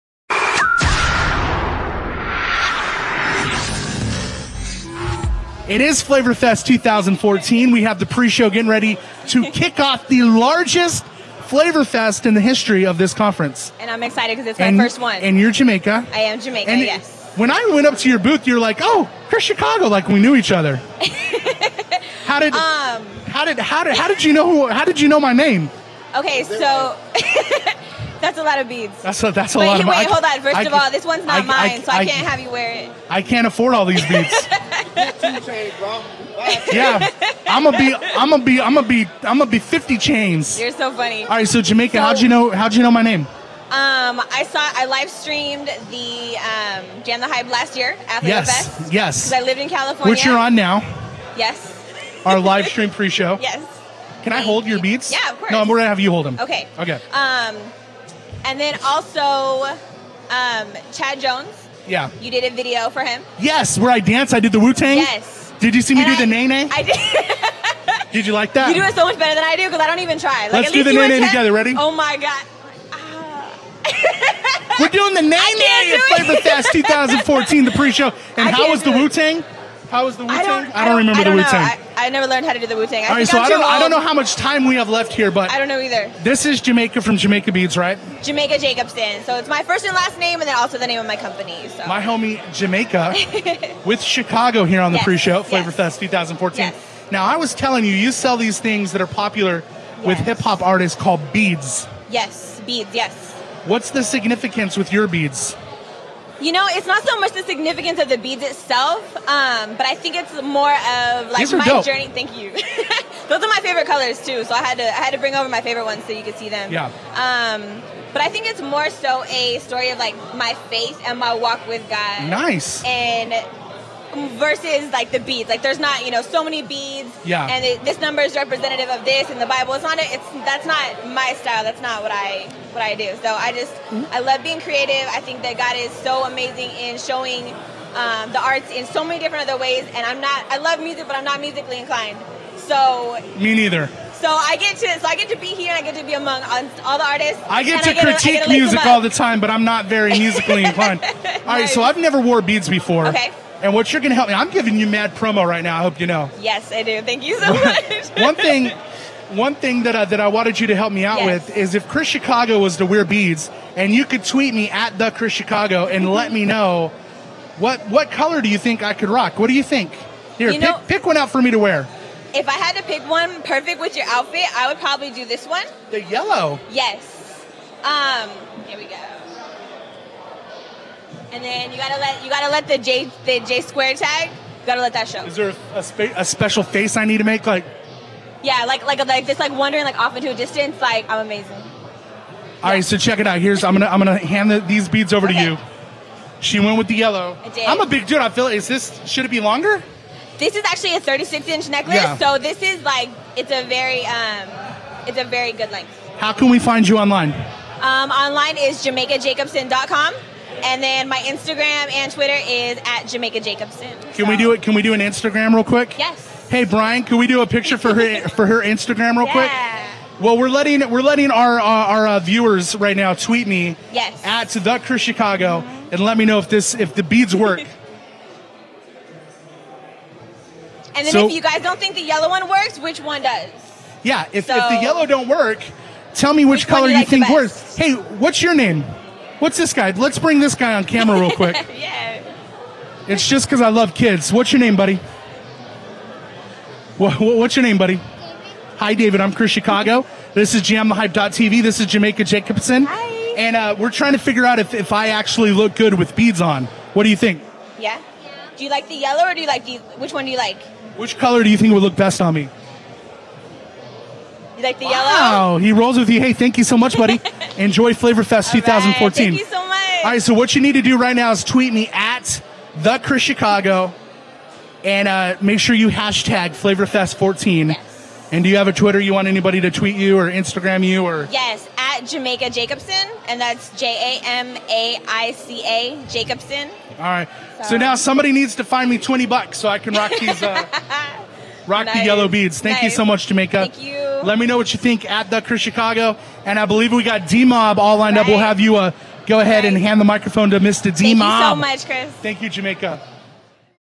Flavor Fest 2014: Jamaica Jacobson Interview [@JamaicaJacobson FlavorFest2014 @JamTheHype]
Chris Chicago spends a minute with jewelry designer Jamaica Jacobson to discuss beads and more during our Flavor Fest pre-show Thursday night from Flavor Fest 2014!  Check out the interview right here on Jam The Hype’s YouTube!